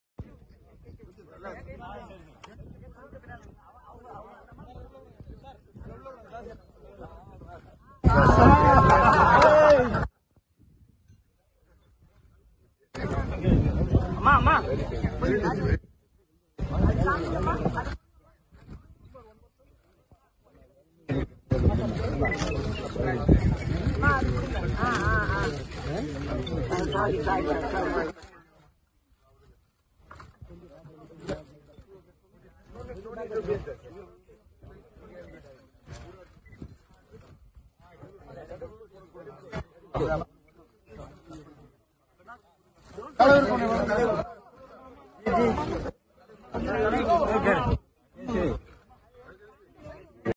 Rahul Gandhi Eating Nungu (Palm Sweet)
Rahul Gandhi Eating Nungu while campaign in elections